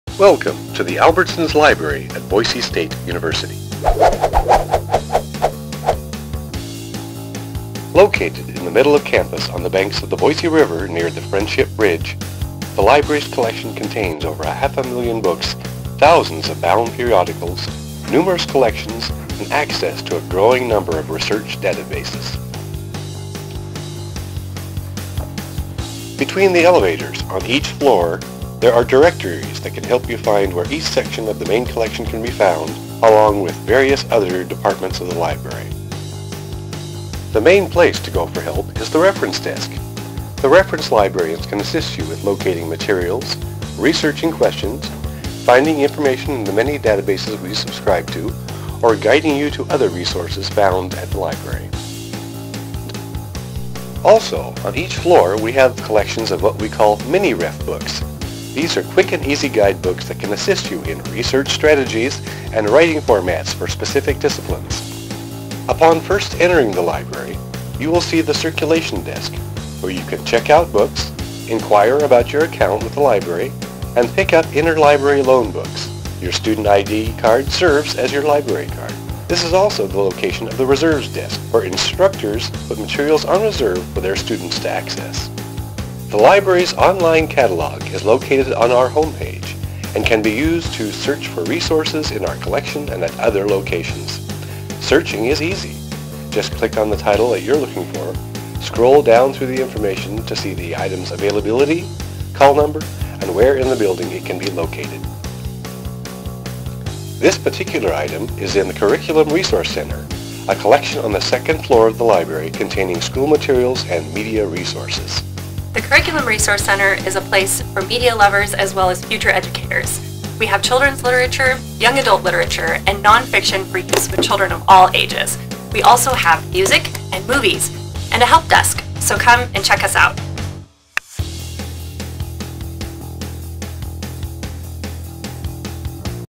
Library Tour
A first run at creating a tour video for the library.  It's been fun.  Ironically, I'm struggling to find a good way to do sound recording at work.  The quality of the narration on this one is making me cringe.  Now I may have to completely reshoot this as I was told that we ought to get a better camera if we're going to produce something like this.  In the big picture, that's kind of nice.